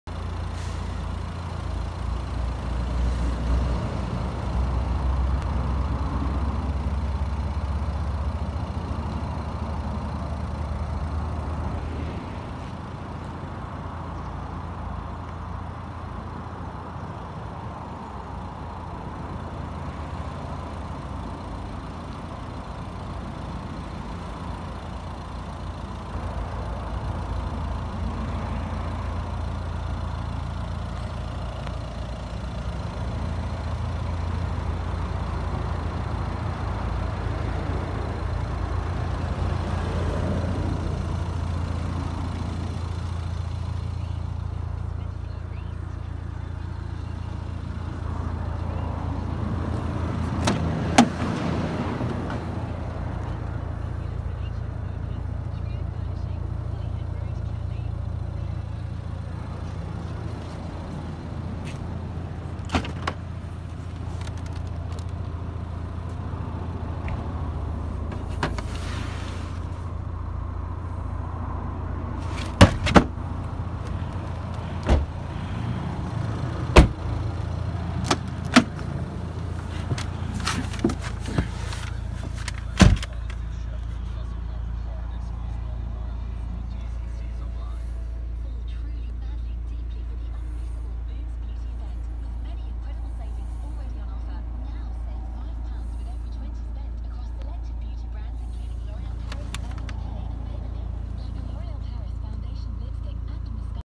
Volvo S60 at auction, direct from main dealer group
In our Timed Online auction on Tuesday 18th August 2020, Direct from Greenhous main dealer group. Bidding and registration open at 10am on Monday 17th August. for further details. Happy bidding!!